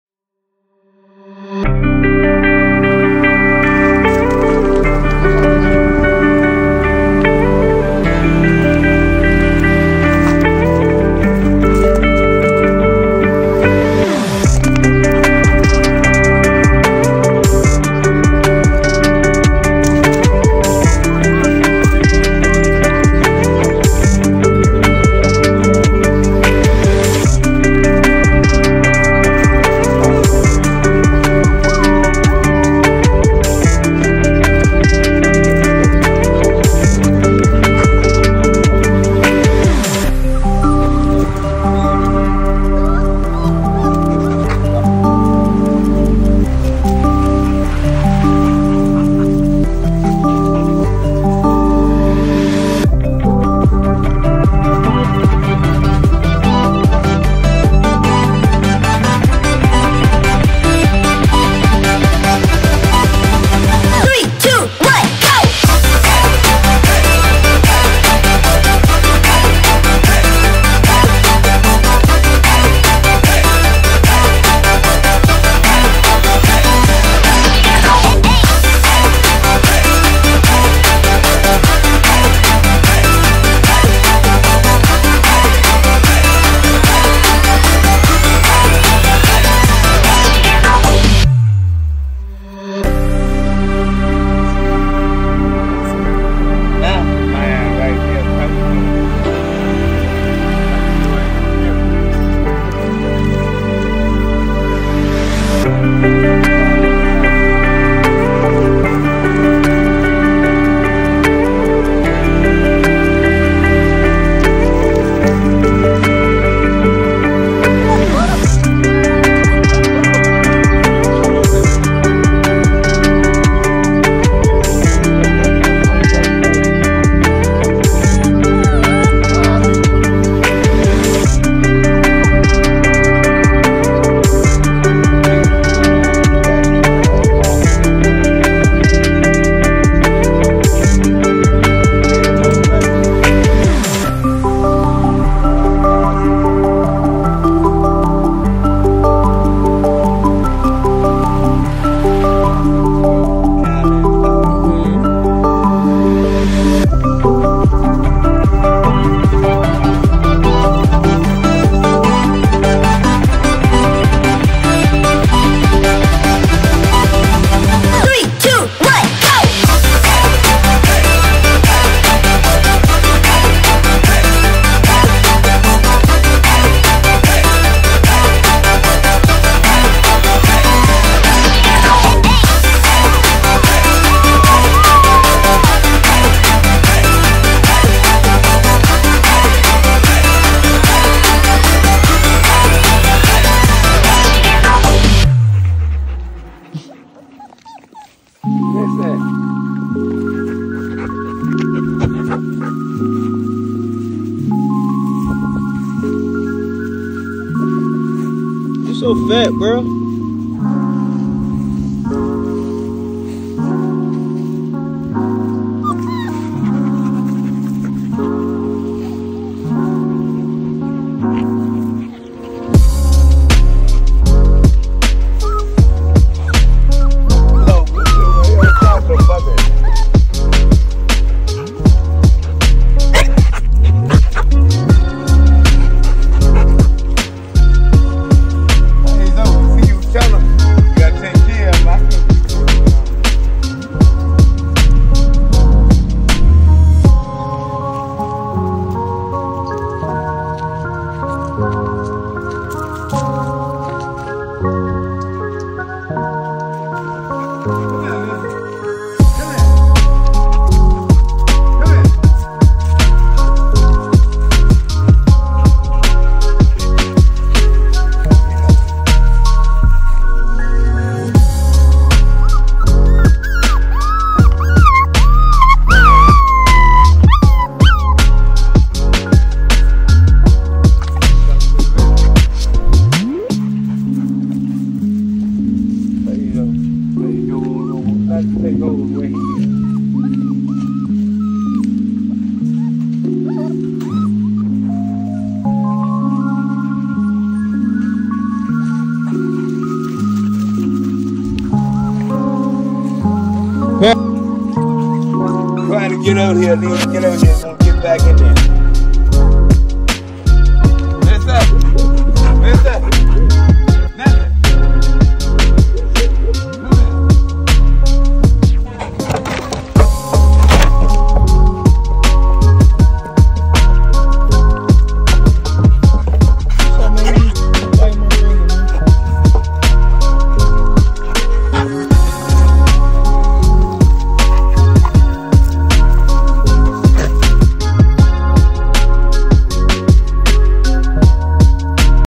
DayInTheLifeW/ (zbkennel). Last play day before pups go to there new home 🏡 Pt69.
Musician: Mike

Musician: Cheel

TikTok (zbkennel).
Facebook (zbkennel).
Twitter (zbkennel).